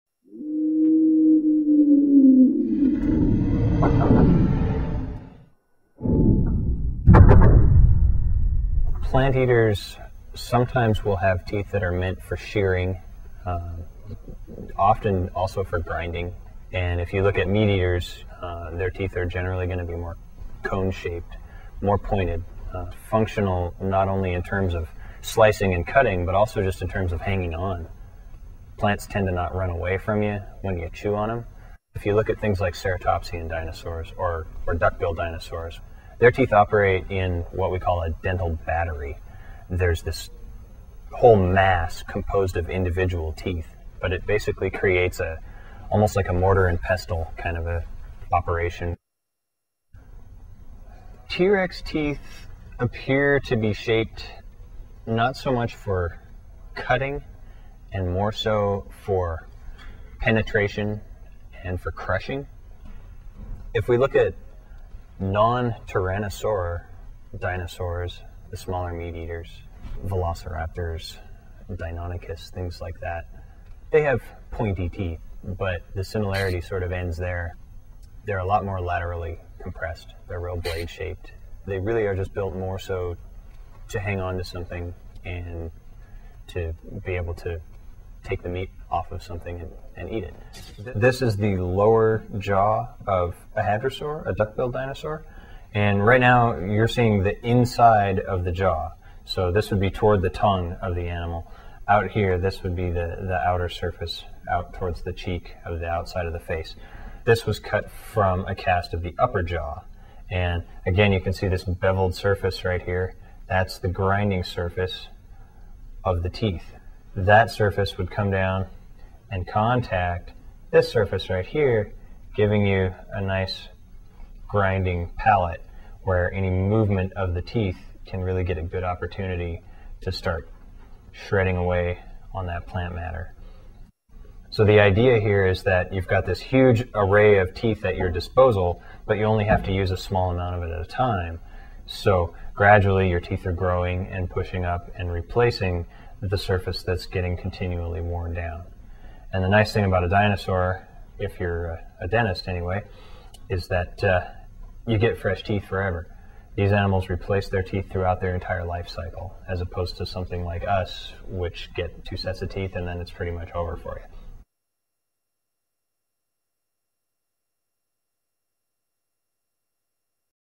Dinosaur Teeth
Plant-eating and meat-eating dinosaurs have different kinds of teeth used in different ways.